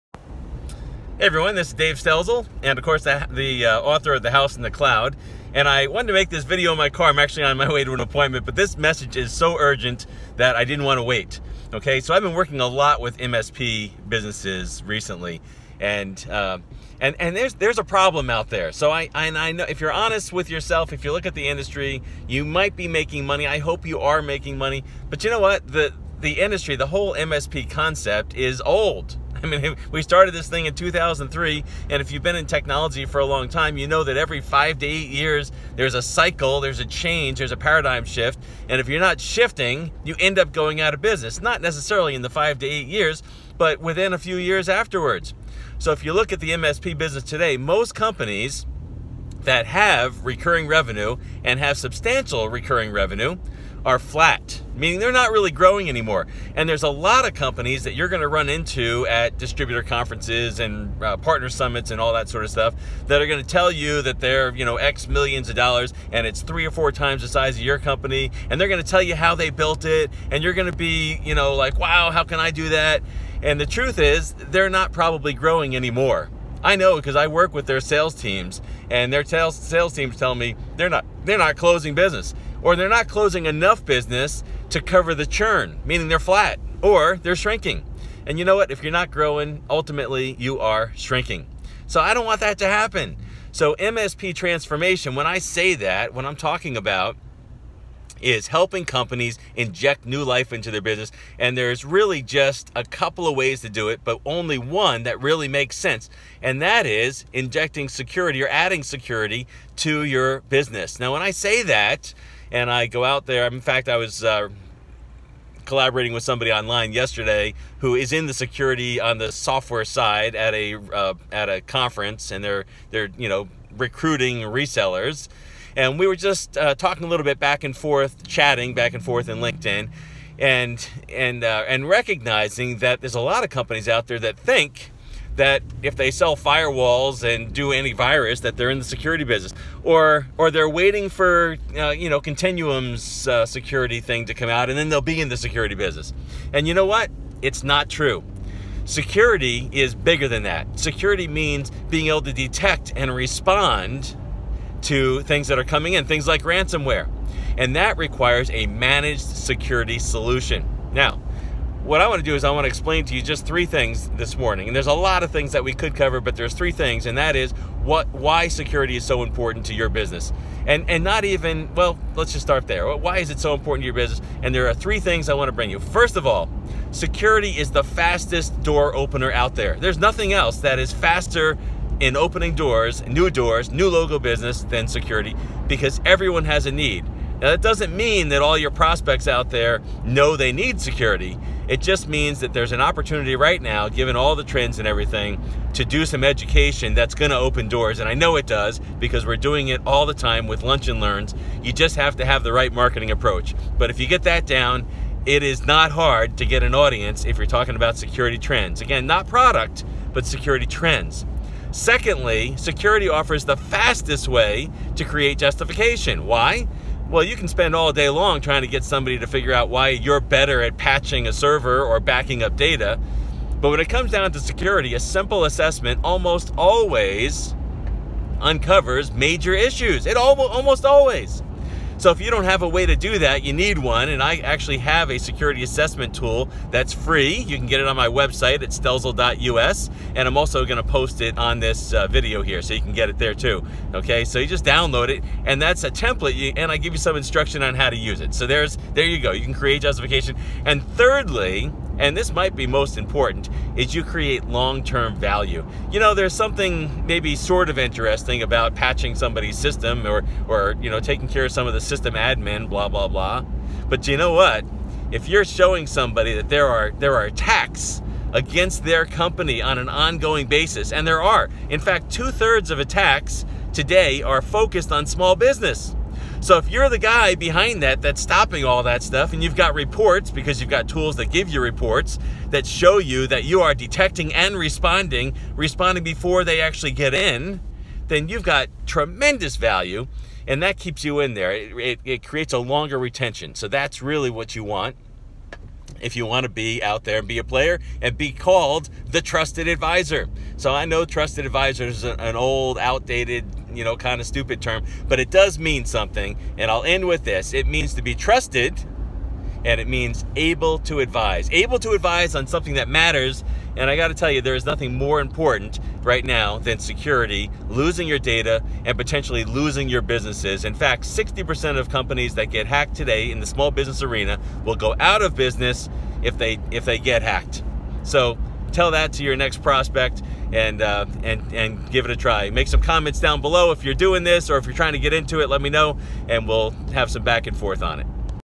Now is the time to transform the MSP business with Security
Transforming The MSP business... it's no longer an option. MSP business is a commodity, but security isn't. In this short video I explain 3 things you get when you invest in security... You can also download my free Riskassessment template right